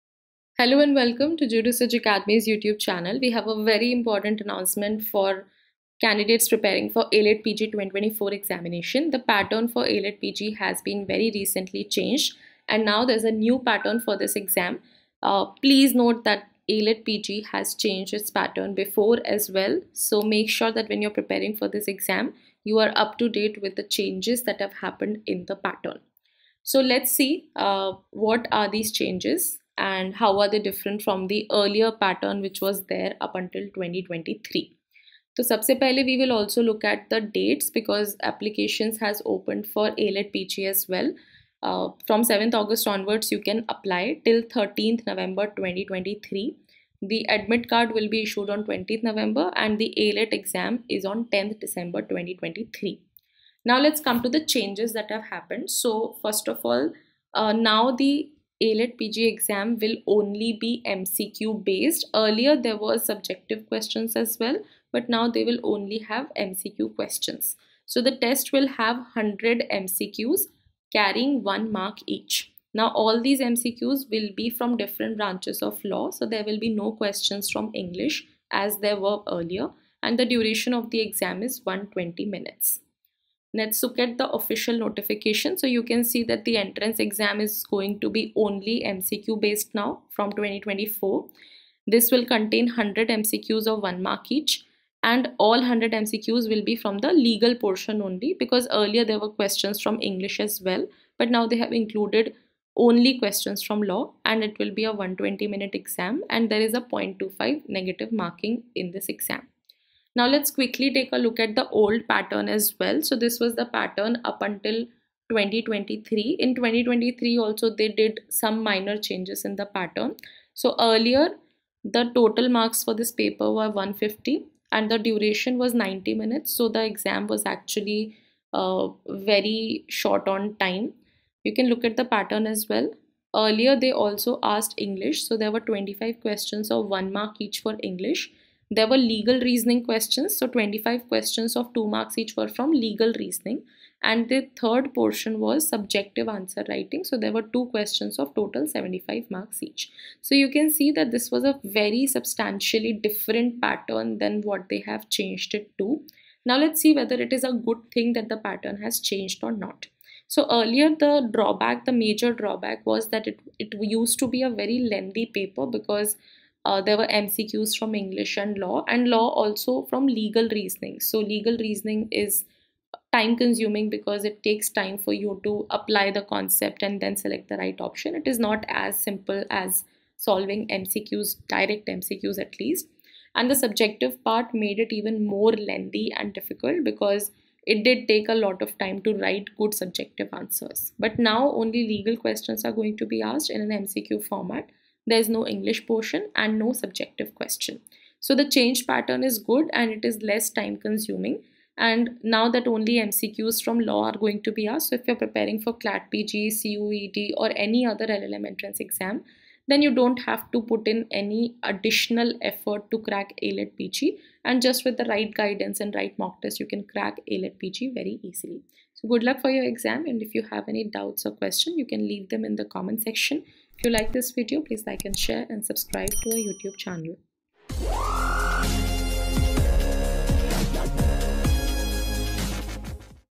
AILET PG 2024- Pattern changed AGAIN!!! Big Update!
AILET has yet again changed the pattern for its PG/LL.M Entrance exam for 2024. Watch this big update here.

For any query regarding LL.M and UGC Courses offered by Jurisedge Academy, Whatsapp Now @ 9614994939